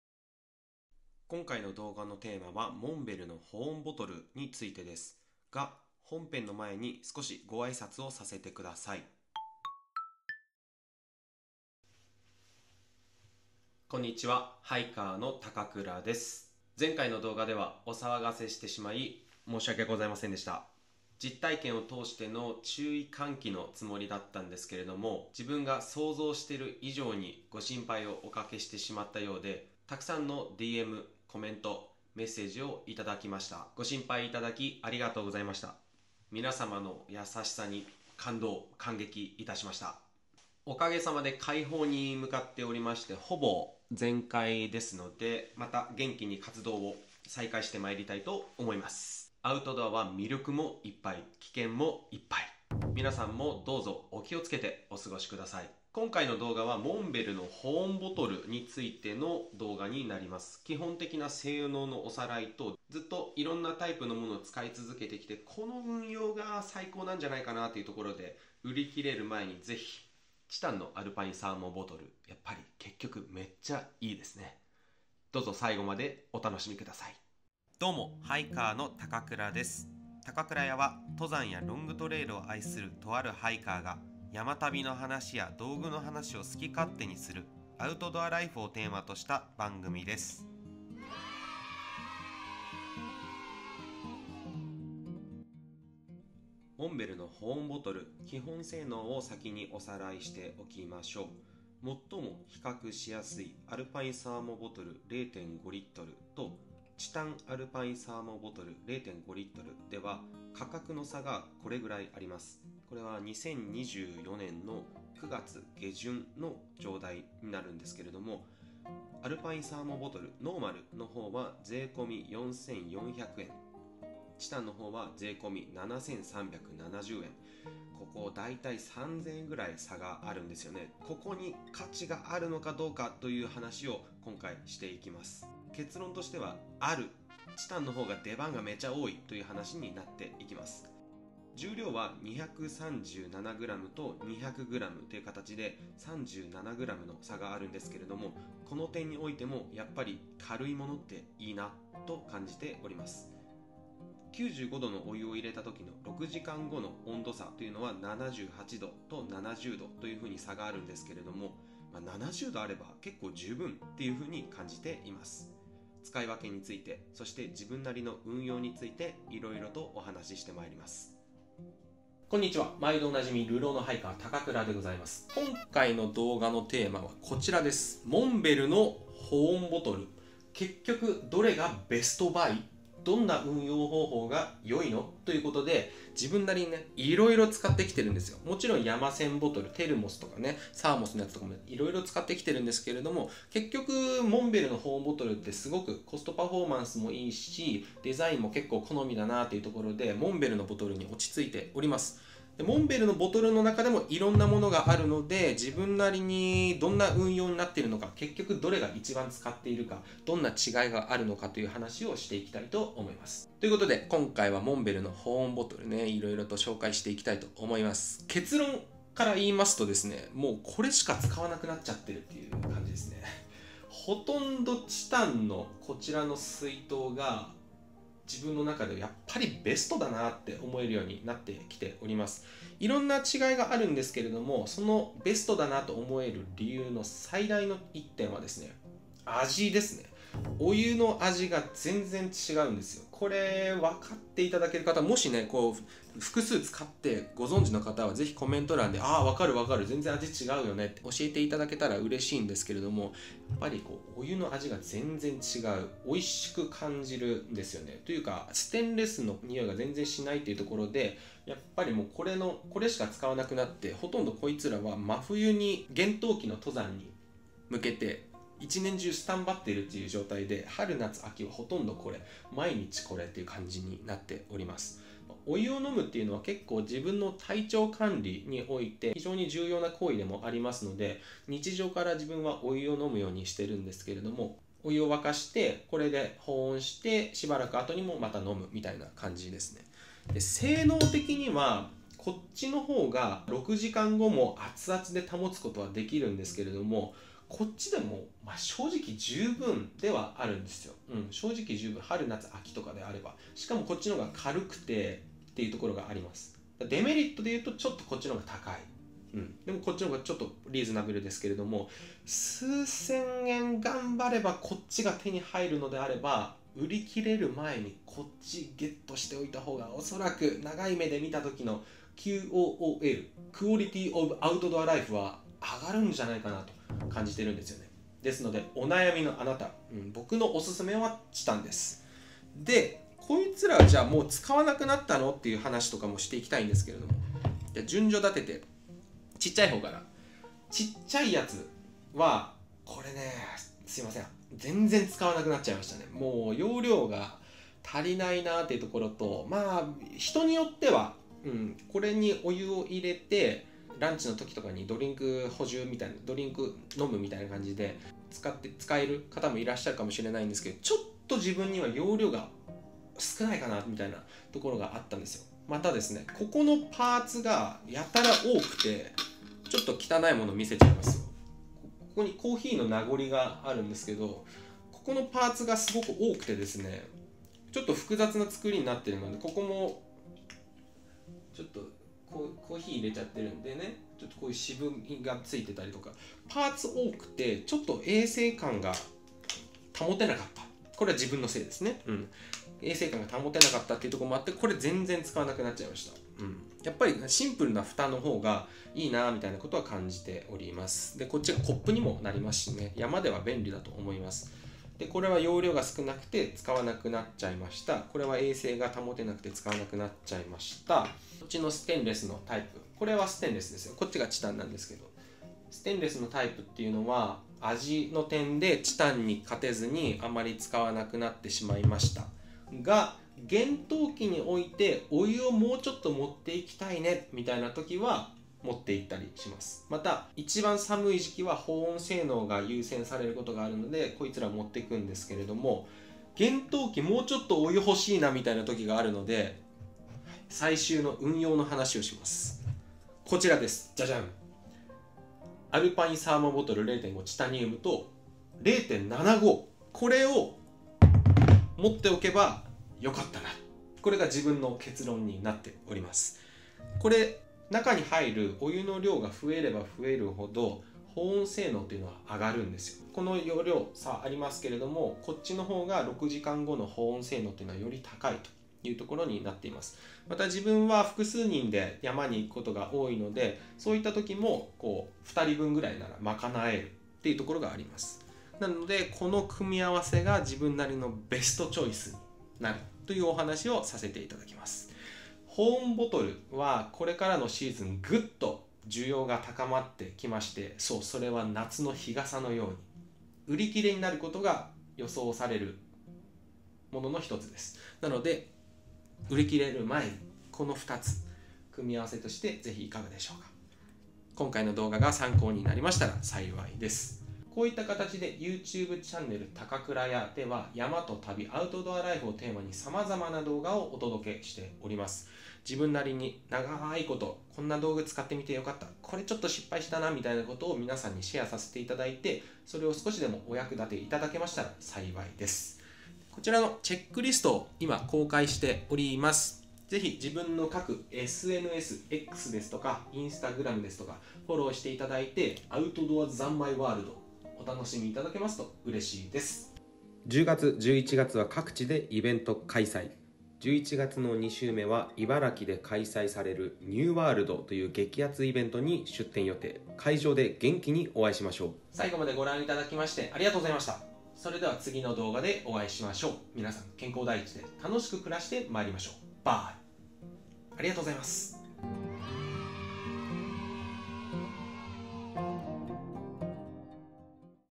【山の名品紹介】チタン？ノーマル？モンベル『アルパインサーモボトル』私的結論と最終運用
⚫︎モンベルの保温ボトルについて語りました！ニトリやサーモスなどいろいろ使ってきましたが、買うならこの組み合わせがオススメです…という経験からのお話です。

⭐︎イベントの詳細

⚫︎11/16 MATSUMOTO TRAIL DAY

⚫︎登山装備チェックリストはコチラから

⚫︎もくじ
0:00 回復のご挨拶
00:55 保温ボトルの話
01:44 基本性能のおさらい
03:51 結論①
05:49 使わなくなったもの
07:30 結論②